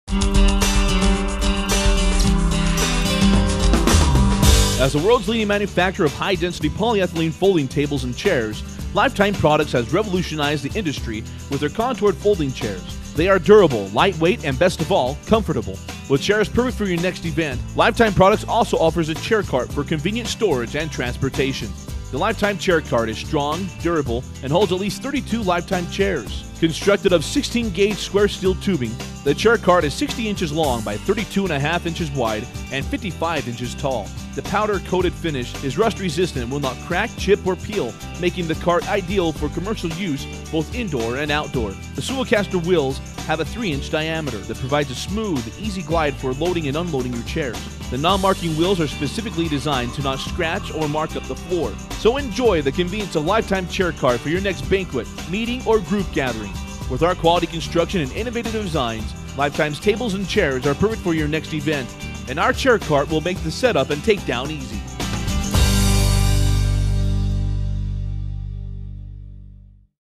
Lifetime Chair Cart
The Lifetime Chair Cart allows you to transport and store up to 32 Lifetime folding chairs. Heavy-duty steel construction with gliding swivel caster wheels.